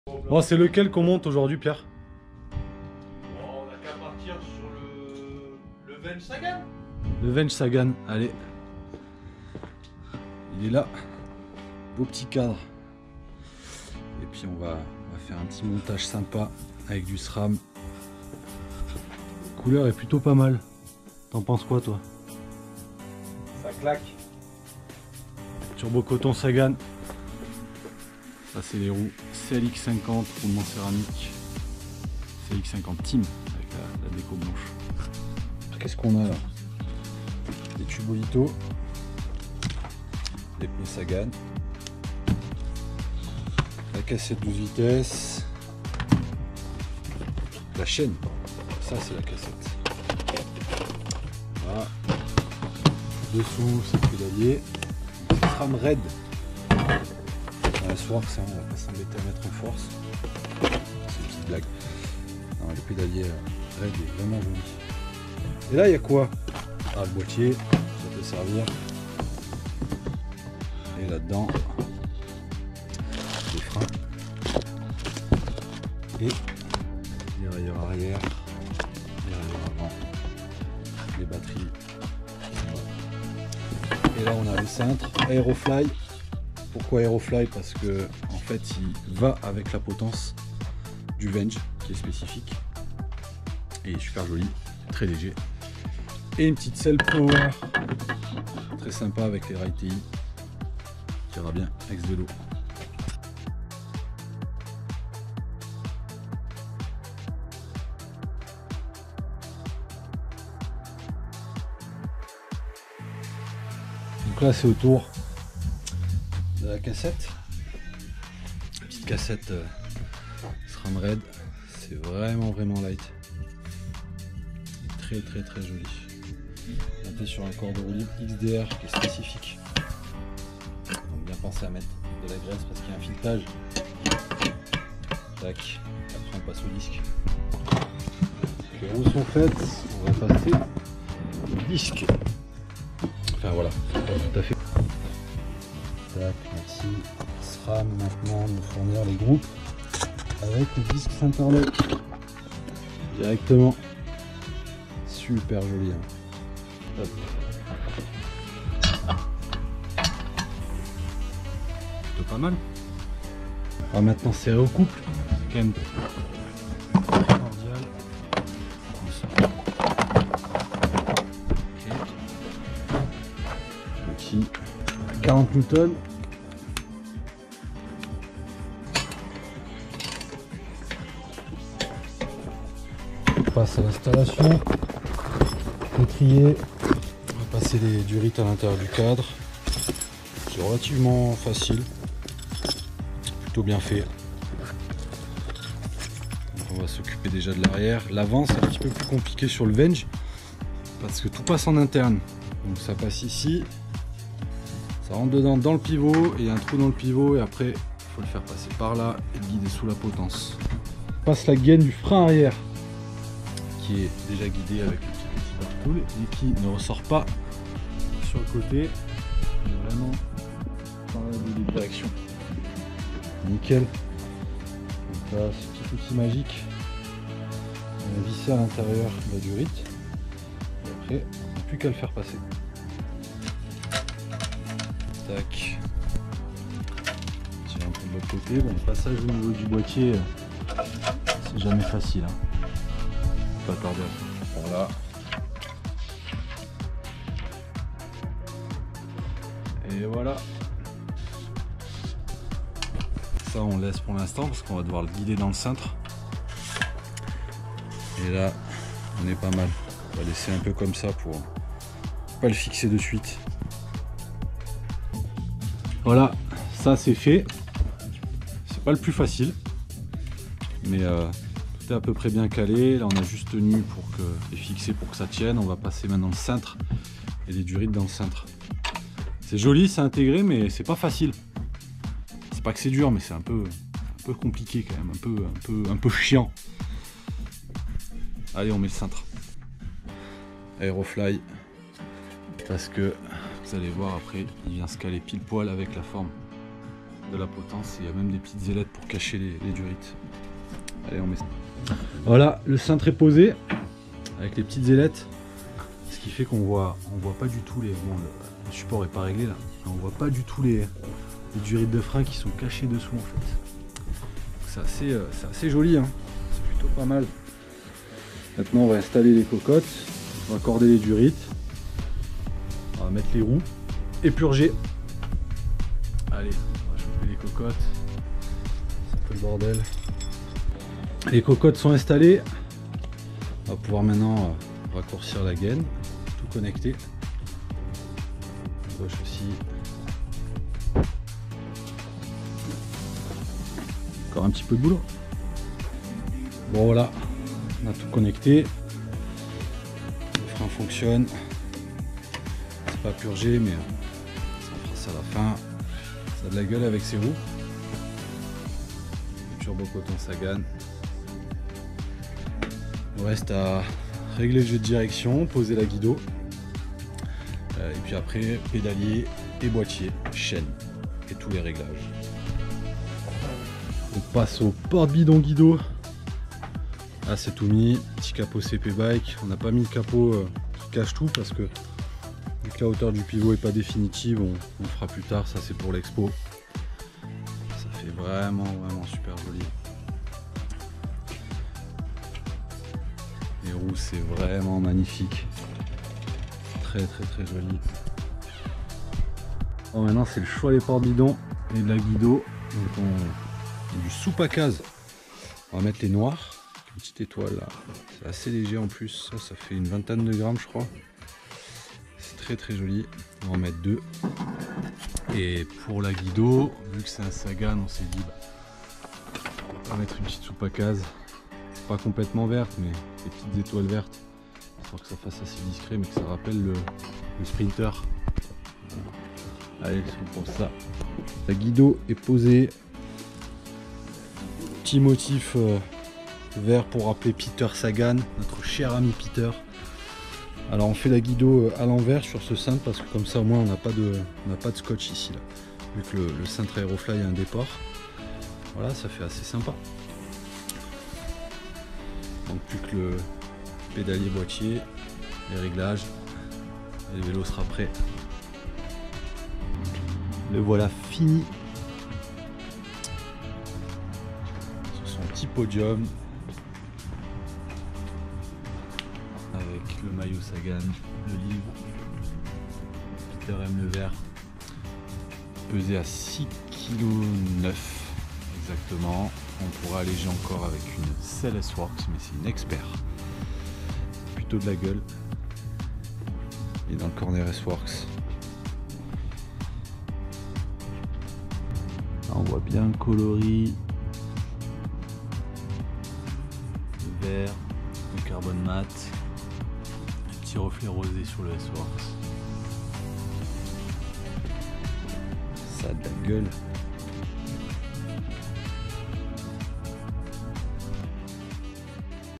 Montage d'un Specialized Venge SWORKS Peter Sagan édition ! 250 exemplaires dans le monde !
Montage d'un Specialized Venge édition limitée PETER SAGAN avec un groupe Sram RED Etap AXS 12v et des magnifiques roues ROVAL CLX50 Team.

Des questions ? RDV dans la section commentaires !